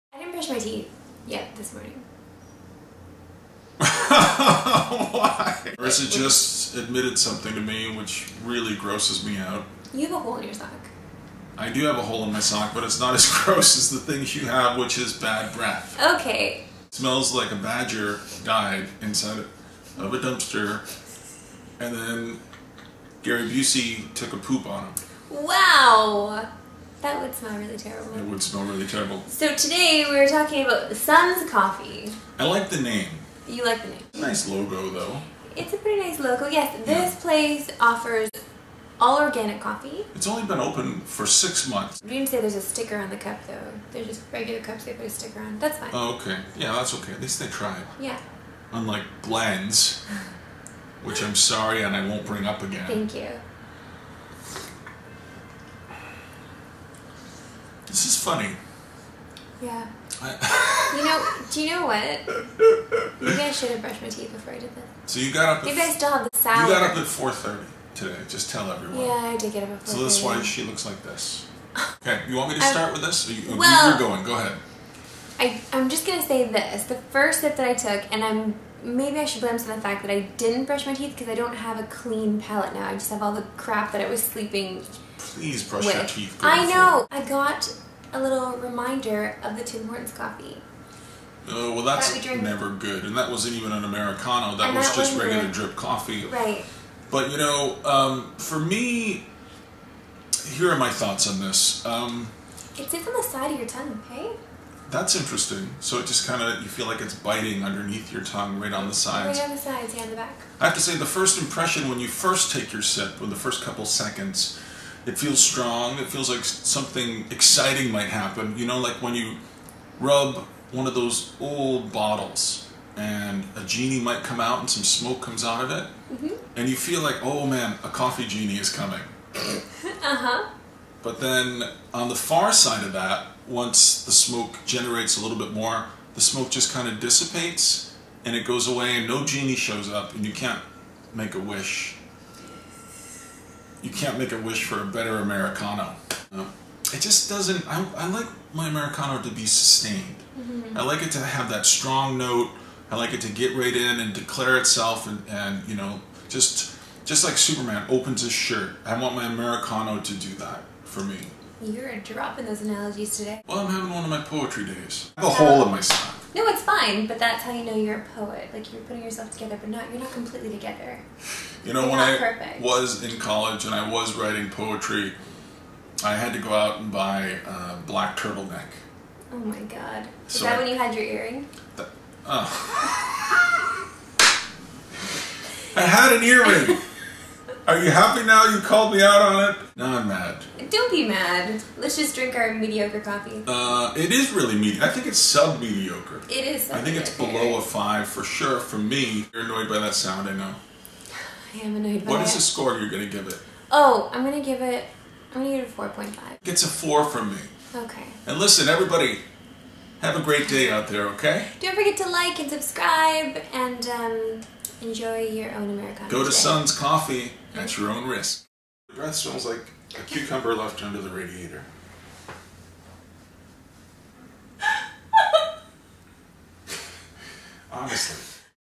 A**holes and Americanos Ep 7: Sun's Coffee
Americano reviews with Scott C Jones. Yuuuup, only a-holes would do something like this.